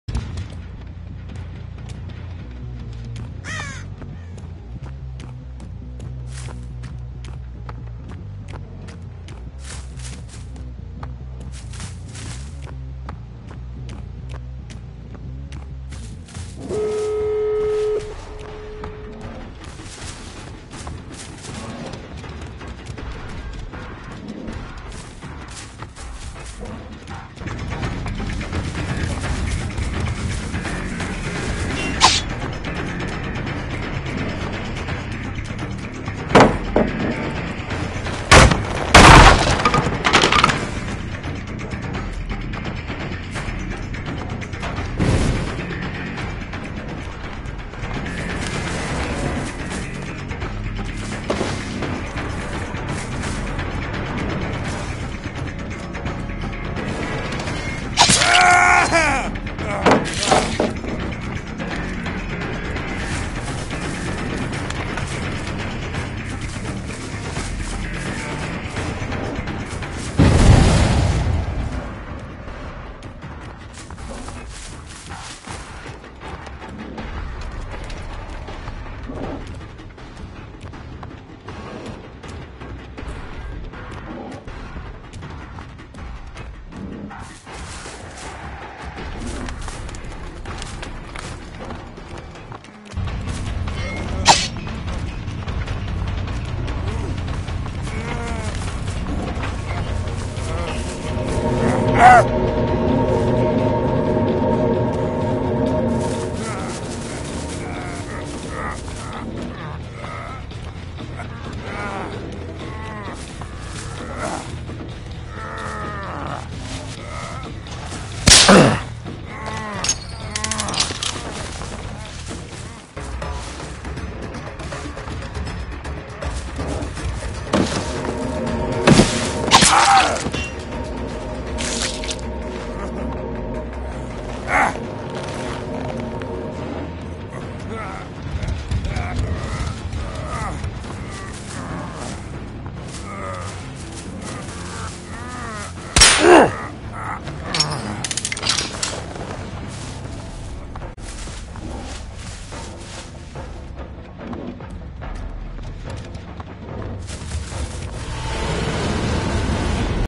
Two end-game trap catches smh
The last chance for another kill gets away twice because I step in my own trap TWICE.  THE. SAME. TRAP.